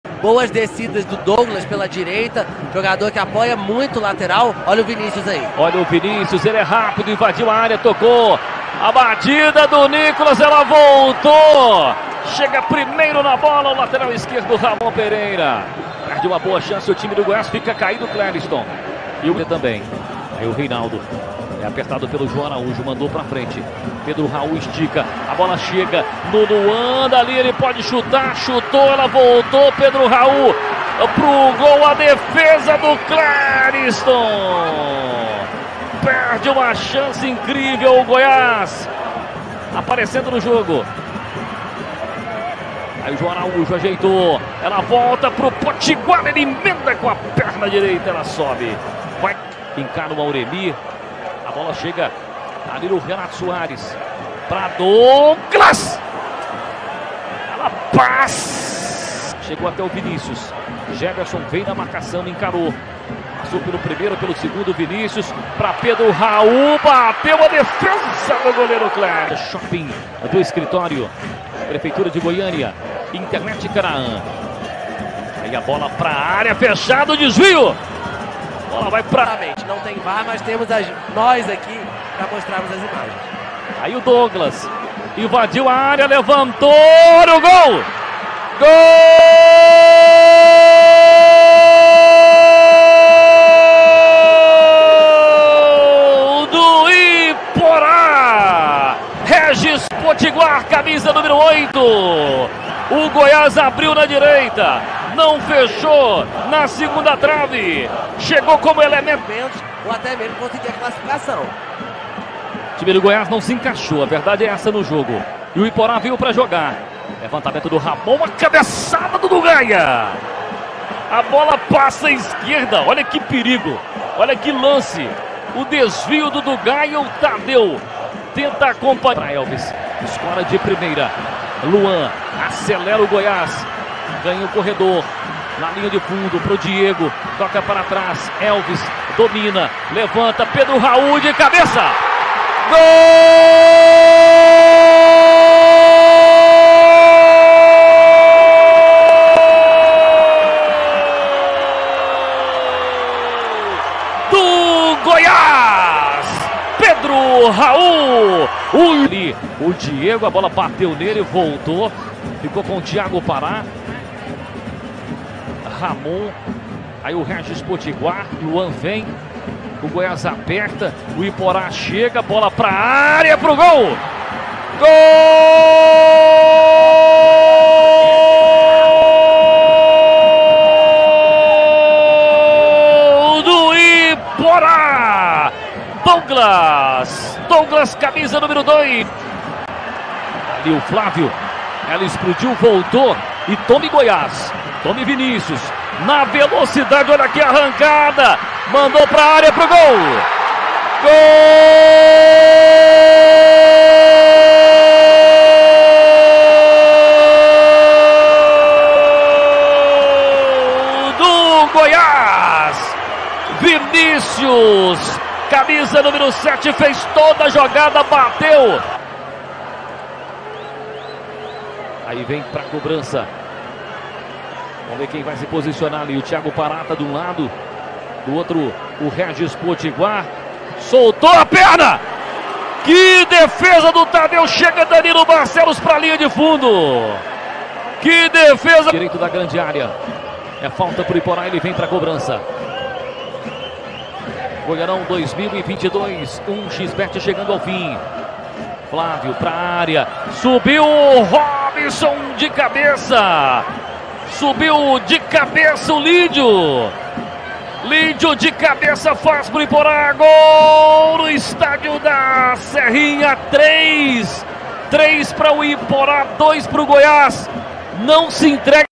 Goiás 2 x 3 Iporá, semifinais, jogo de volta, Goianão 2022, Goiás classificado
Goiás 2 x 3 Iporá, semifinais, jogo de volta, Goianão 2022, Goiás classificado para a final do Campeonato.
Gols de Régis Potiguar, Douglas e Lídio para o Iporá.
Robson (contra) e Vinícius para o Goiás.